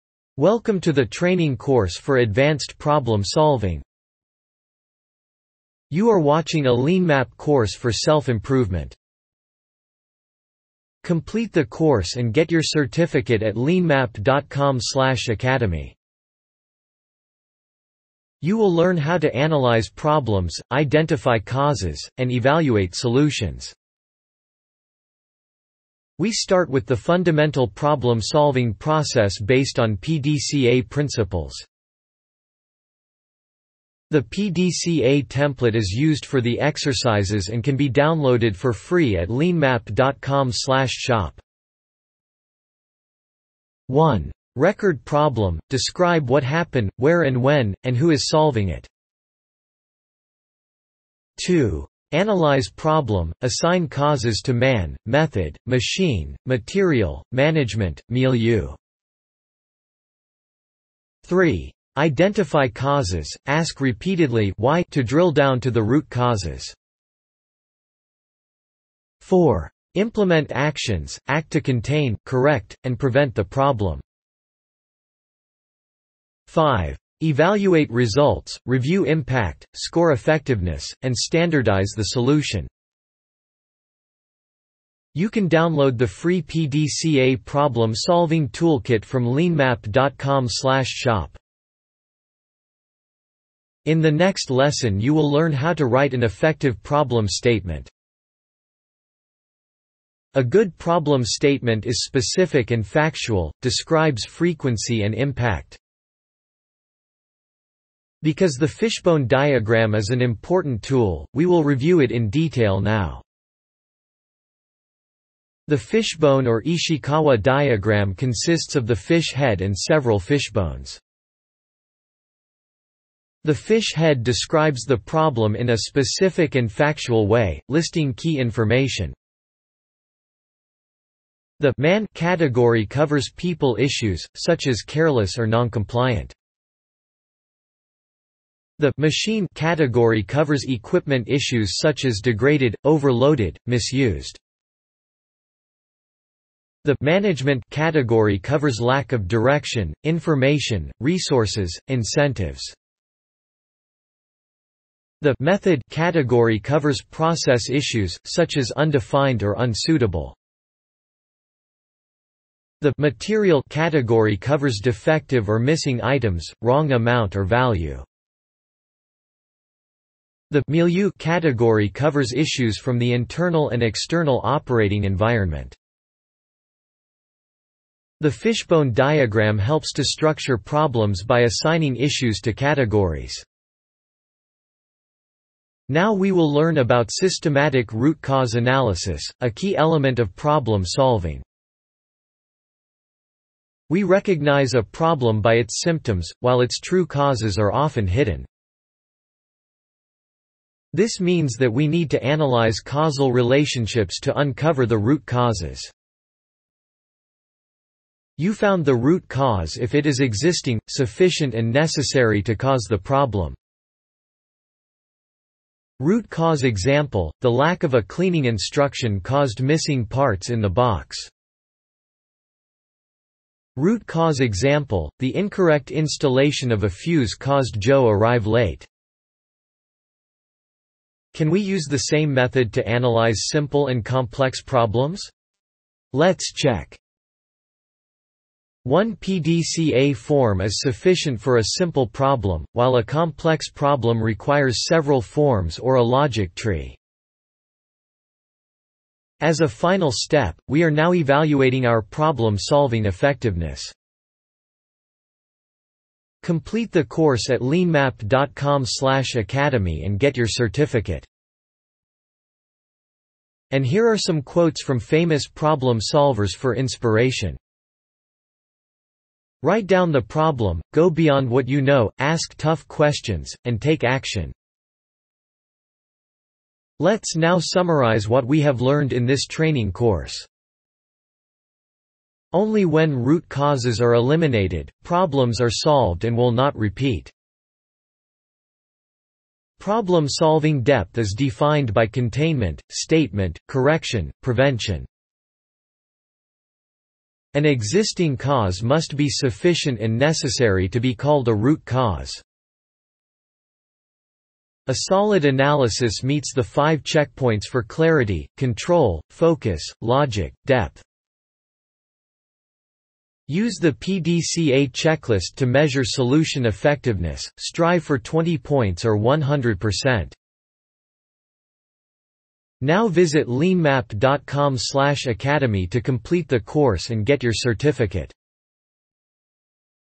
Problem Solving Course: 5 Steps to Eliminate Root Causes with PDCA, 6M, 5W
This video explains how to solve problems in 5 steps by eliminating Root Causes, using the PDCA Process, 6M Fishbone Diagram, 5-Why Analysis, and 3W Action Plan. The video also provides links to free resources (download the PDCA Toolkit) and training course (become a Certified Problem Solver).